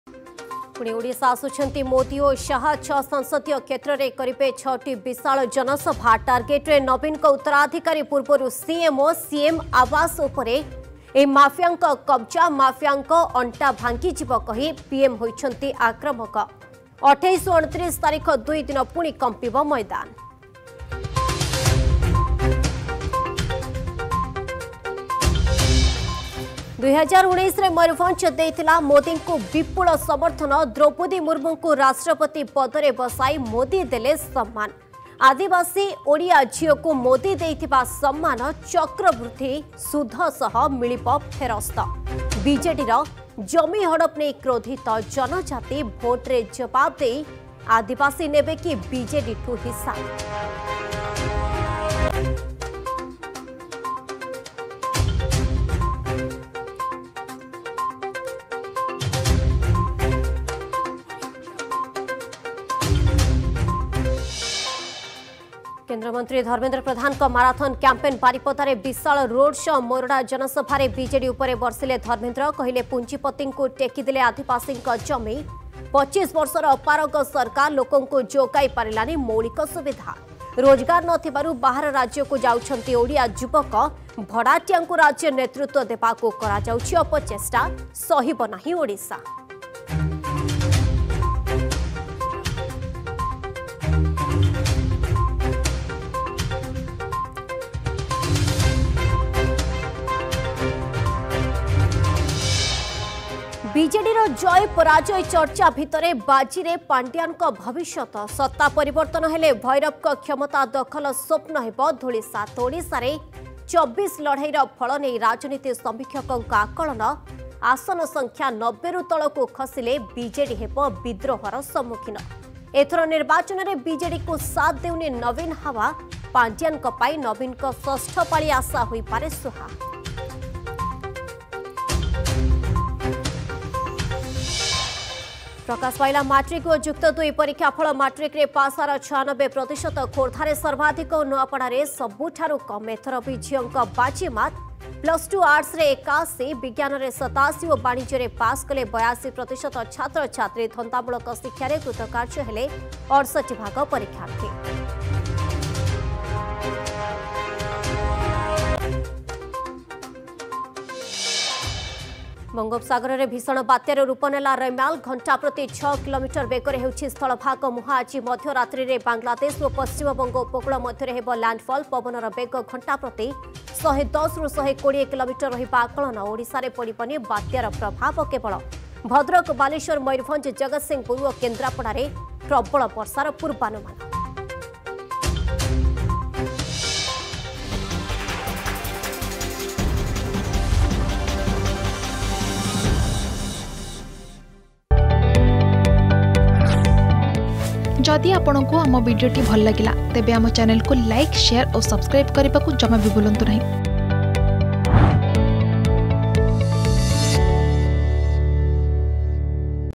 Headlines @ 7 Pm || 26 May 2024 || Argus News
+ ପୁଣି ମୋଦି ଓ ଶାହ - କମ୍ପିବ ମଇଦାନ
+ ମୟୂରଭଞ୍ଜ-ଆଦିବାସୀ-ବିଜେଡି ହିସାବ
+ `ଭଡ଼ାଟିଆଙ୍କୁ ନେତୃତ୍ବ, ସହିବନି ଓଡ଼ିଶା'